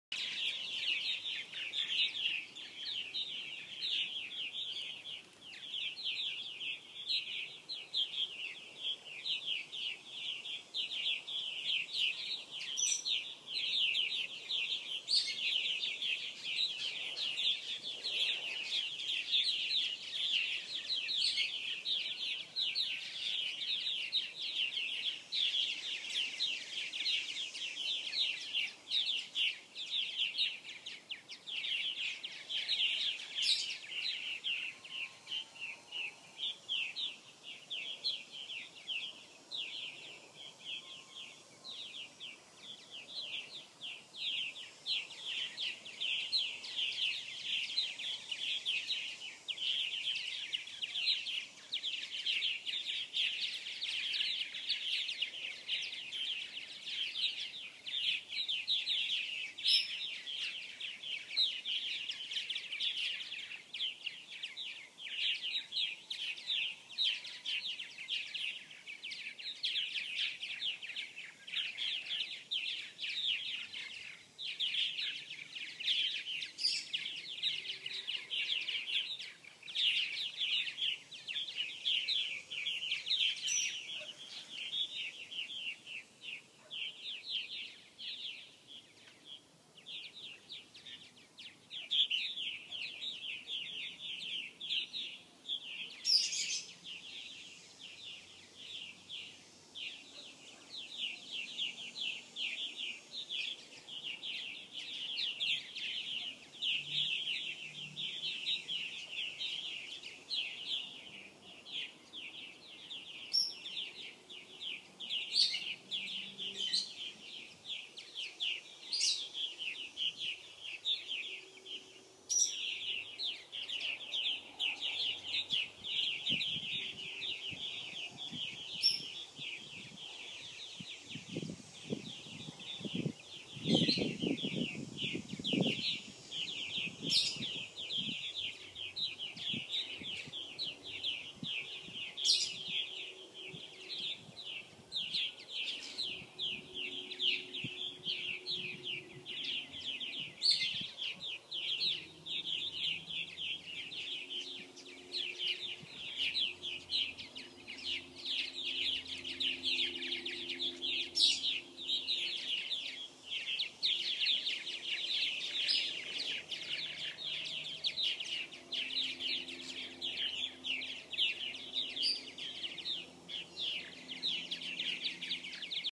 Purple Martin Colony 2014
A short clip of our purple martin colony in South Florida. All gourds, poles, and houses can be found at PurpleMartins-R-Us.com or PurpleMartinPlace.com.